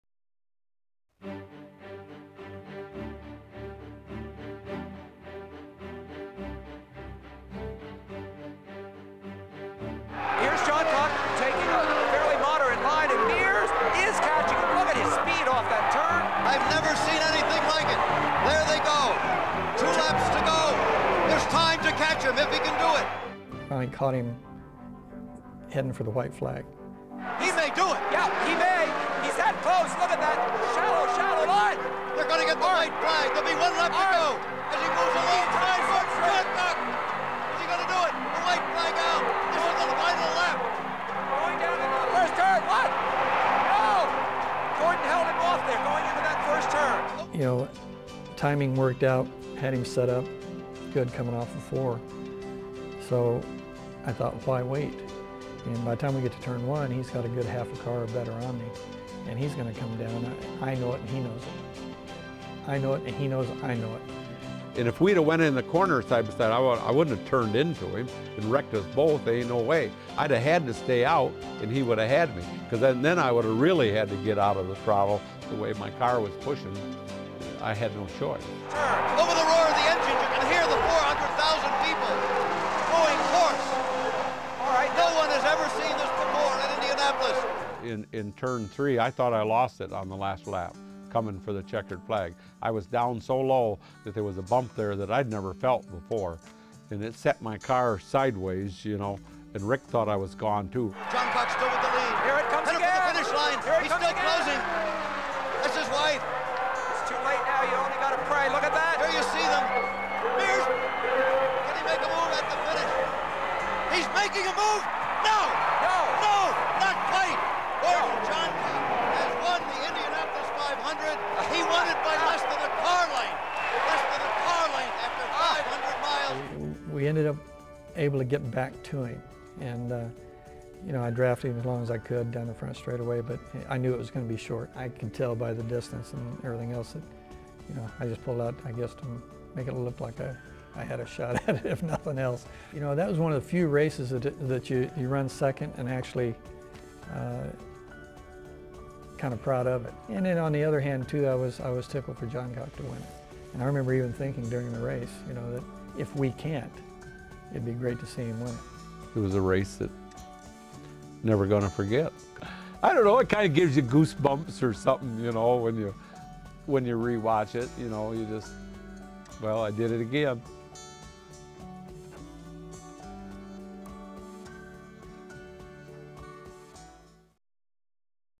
Johncock on 1982 Centennial Era Moments
Gordon Johncock talks about the 1982 Indianapolis 500 in this episode of Centennial Era Moments by the Indianapolis Motor Speedway